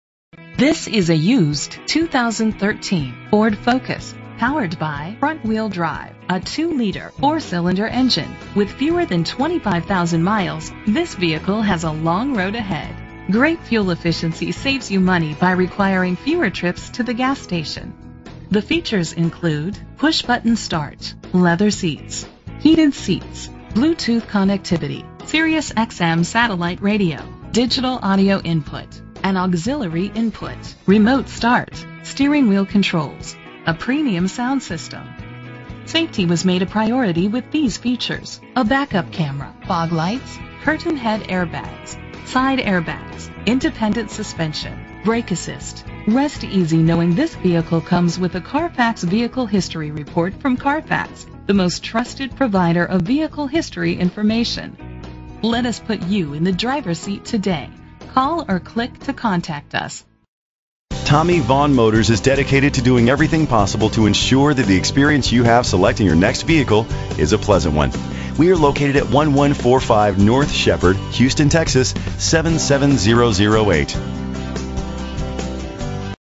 2013 Ford Focus - Houston TX
Phone:
Year: 2013
Make: Ford
Model: Focus
Trim: Titanium
Engine: 2.0 liter inline 4 cylinder 16 valve
Transmission:
Color: Ingot Silver Metallic
Mileage: 22142
Address:
1201 Shepherd Drive